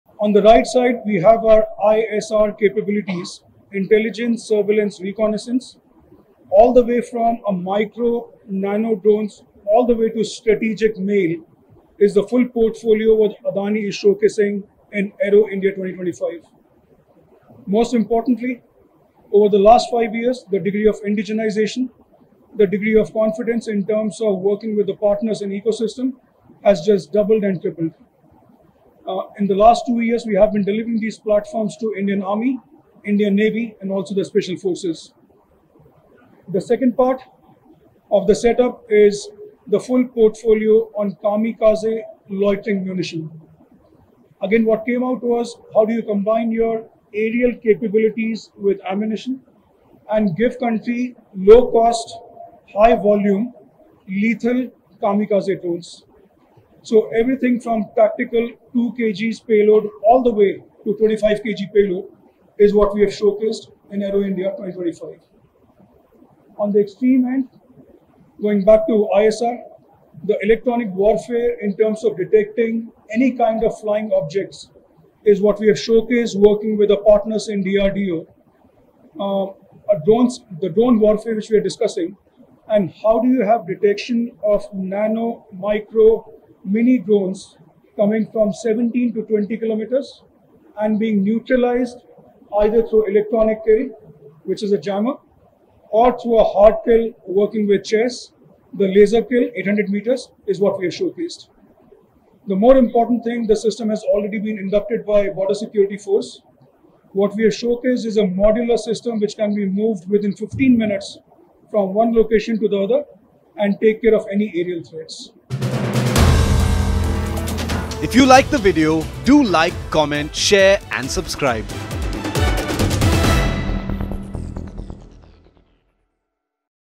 Aero India 2025 | Adani Defence, DRDO Unveil Vehicle-Mounted Counter-Drone System
Adani Defence & Aerospace, in collaboration with DRDO, has unveiled India’s Vehicle-Mounted Counter-Drone System at Aero India 2025. Launched by Dr. B K Das, Director General (Electronics & Communication System) of DRDO, the system marks a major advancement in India's defence against evolving aerial threats. Designed for precision and agility, it features a high-energy laser, a 7.62 mm gun, and advanced sensors for real-time target tracking and neutralization within a 10 km range. Adani Defence & Aerospace CEO Ashish Rajvanshi highlighted the system’s role in strengthening India’s armed forces, underscoring the success of DRDO’s R&D and the Transfer of Technology framework.